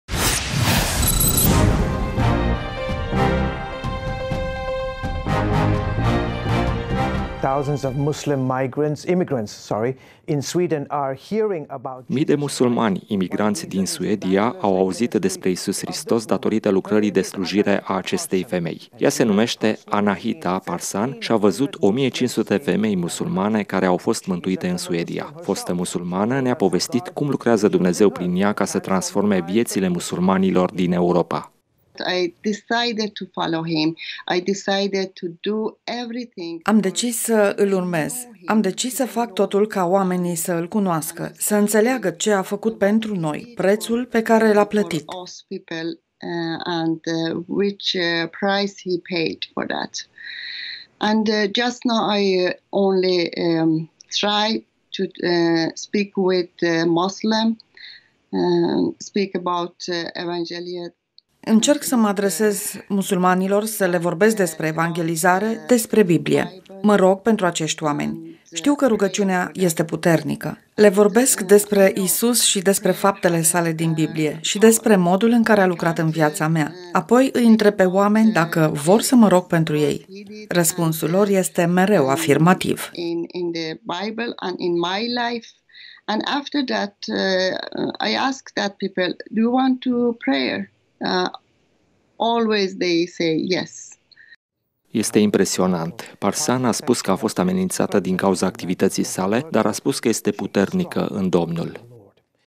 Evanghelista Annahita Parsan vestește evanghelia în rândul musulmanilor din Suedia
Mii de musulmani imigranți din Suedia au auzit despre Isus Hristos datorită lucrării de slujire a acestei femei. Ea se numește Annahita Parsan și a văzut 1.500 femei musulmane care au fost mântuite în Suedia. Fostă musulmană, ne-a povestit cum lucrează Dumnezeu prin ea ca să transforme viețile musulmanilor din Europa.
„Am decis să Îl urmez. Am decis să fac totul ca oamenii să Îl cunoască, să înțeleagă ce a făcut pentru noi, prețul pe care l-a plătit. Încerc să mă adresez musulmanilor, să le vorbesc despre evanghelizare, despre Biblie. Mă rog pentru acești oameni. Știu că rugăciunea este puternică. Le vorbesc despre Isus și despre faptele Sale din Biblie și despre modul în care a lucrat în viața mea. Apoi îi întreb pe oameni dacă vor să mă rog pentru ei. Răspunsul lor este mereu afirmativ.”
Este impresionant. Parsan a spus că a fost amenințată din cauza activității sale, dar a spus că este puternică în Domnul.
Stire difuzata in emisiunea Mapamond creștin 707 - 4 februarie 2018. Ultimele știri creștine